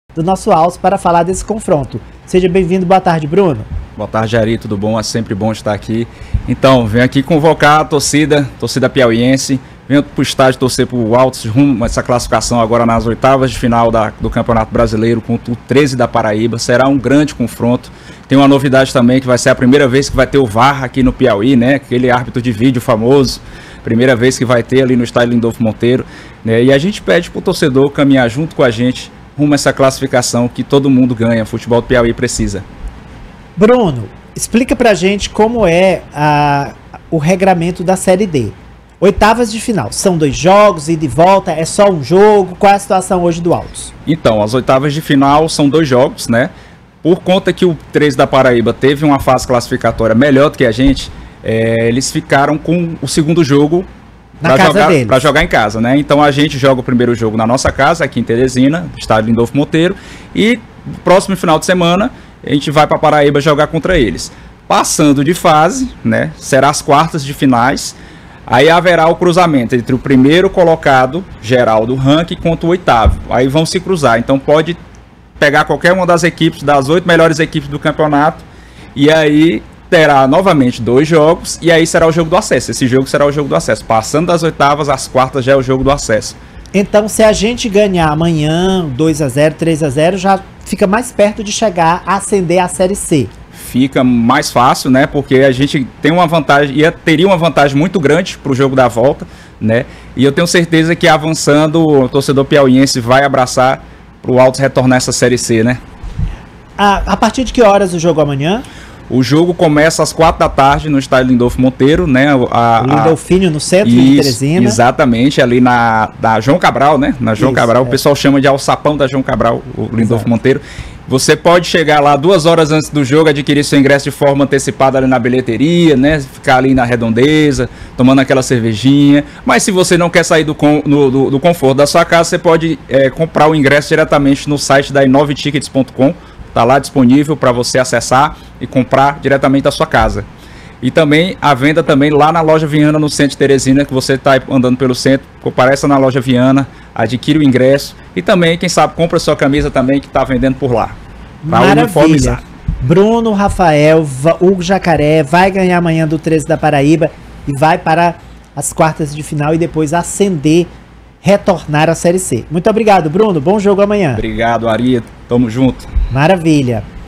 Detalhes sobre o jogo Altos vs Treze do dia 10/08 no Estádio Lindolfo Monteiro
No dia 10 de agosto, Altos e Treze se enfrentaram em um emocionante confronto no Estádio Lindolfo Monteiro. Neste vídeo, trazemos todos os detalhes do jogo, incluindo os destaques das equipes, a análise das táticas utilizadas pelos técnicos e os momentos decisivos da partida. Você vai conferir as jogadas que marcaram o duelo e a performance dos jogadores em campo, além de uma visão sobre o impacto deste resultado na tabela de classificação. Fique por dentro de tudo o que aconteceu neste grande embate do futebol!

Instagram, TikTok e Kwai: @meio_news / @radiojornalfm_